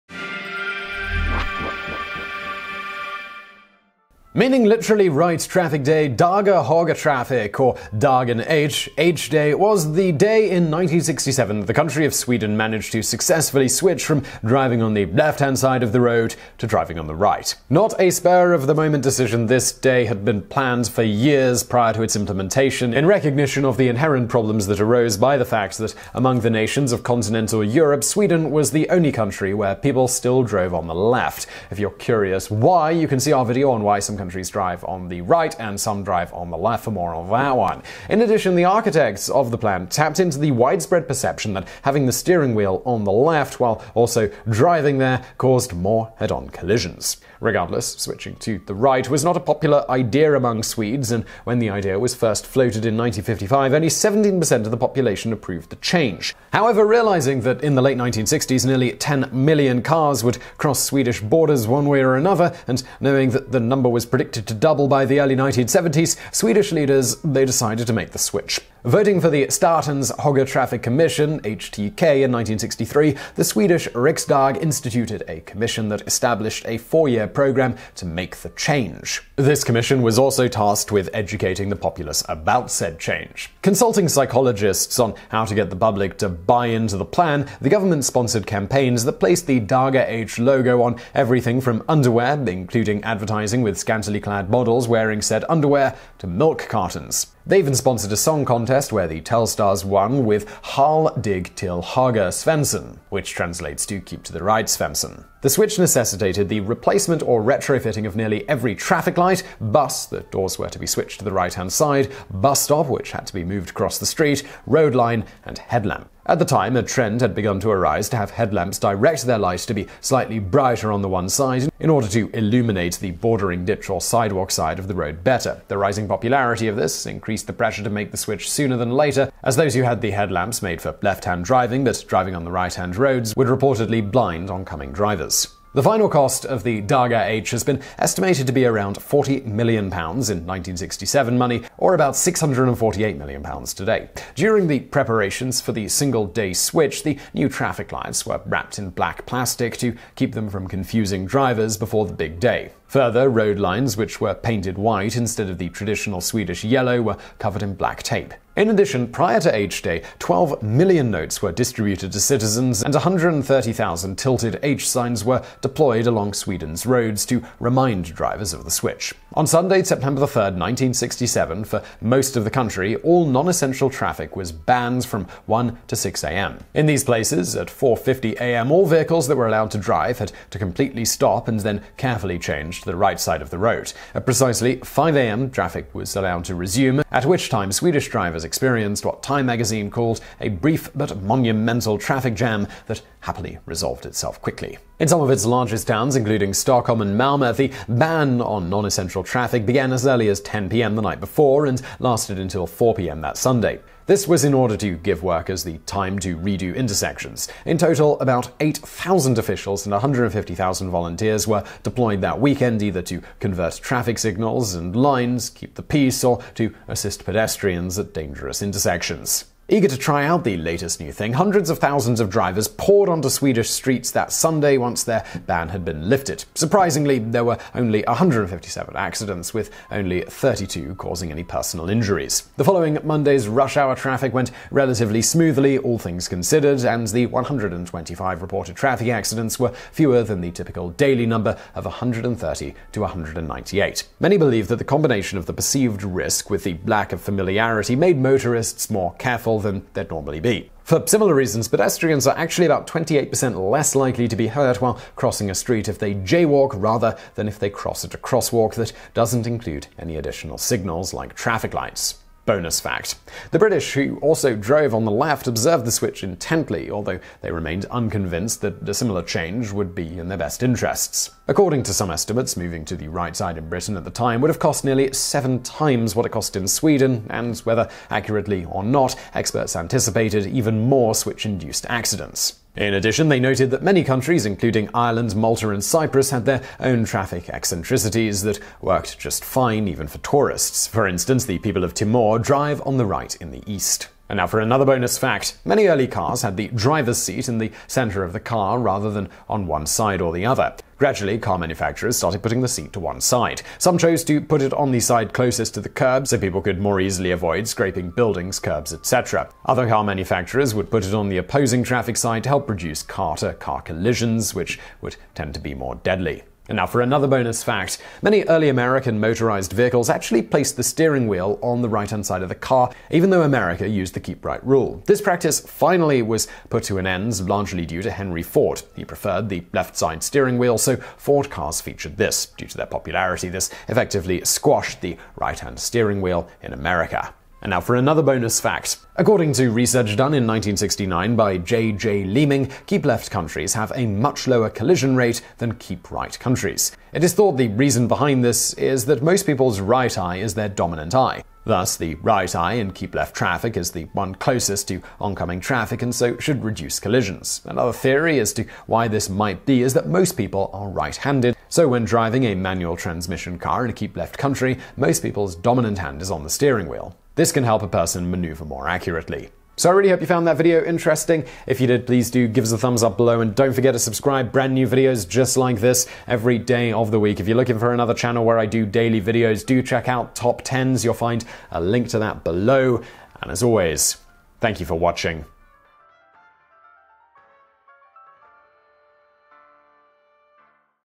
That Time Sweden Changed Its Traffic Direction in a Single Day
More from TodayIFoundOut:

The Truth About Big Ben and the Famous Clock Tower

In this video:

Not a spur of the moment decision, the Högertrafikomläggningen (“The right hand traffic diversion”) had been planned for years prior to implementation, in recognition of the inherent problems that arose by the fact that among the nations of Continental Europe, Sweden was the only country where people still drove on the left. (If you’re curious why, see: Why Some Countries Drive on the Right and Some Countries Drive on the Left.) In addition, the architects of the plan tapped into the widespread perception that having the steering wheel on the left while also driving there caused more head-on collisions.